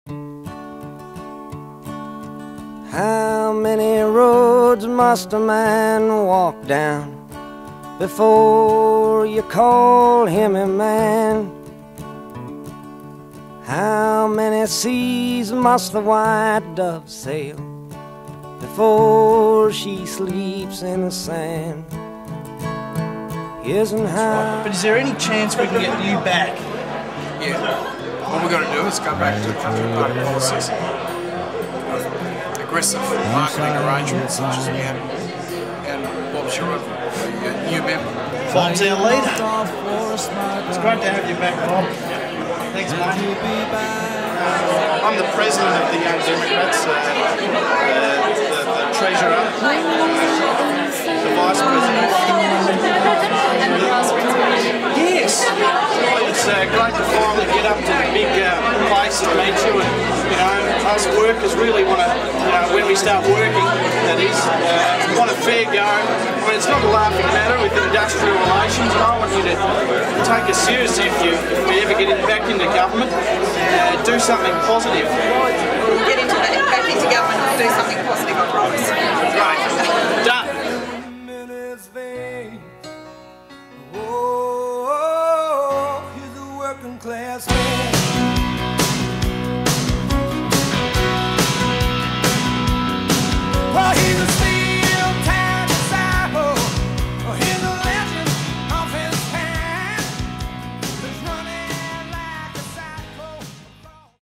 Tad Polls
When each Australian political party's younger versions send their best and brightest stars to tackle the big issues with their heroes in Canberra it results in policy chaos for current Prime Minister Julia Gillard, Independent Bob Katter and former Democrat Natasha Stott Despoja.